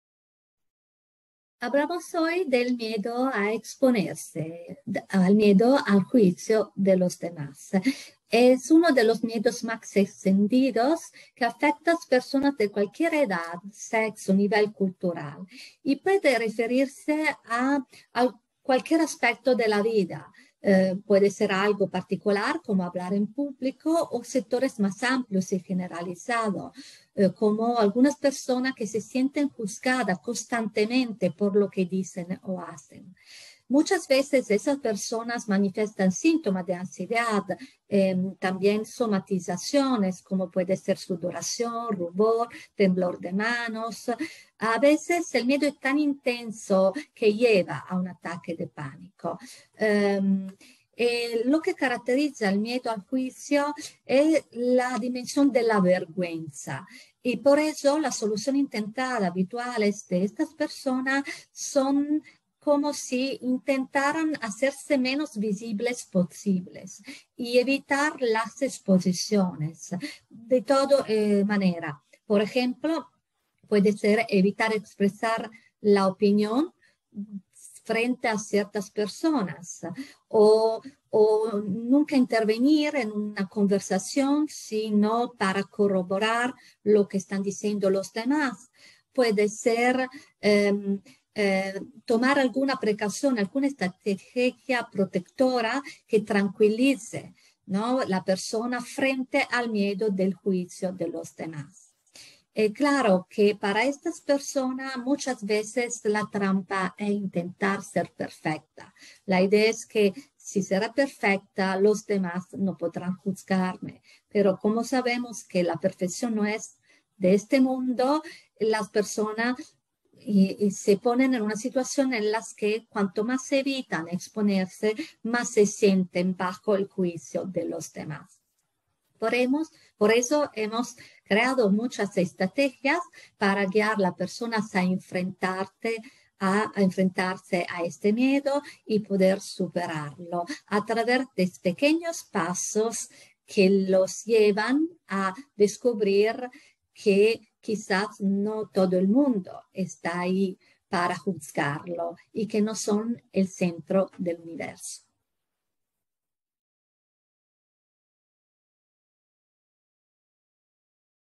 El miedo a exponerse | Roberta Milanese
Roberta Milanese muestra cómo es posible resurgir del insidioso pantano del miedo a no estar a la altura y brinda una serie de recomendaciones fundamentales recogidas a continuación de un único postulado esencial: «La autoestima no se hereda, se construye».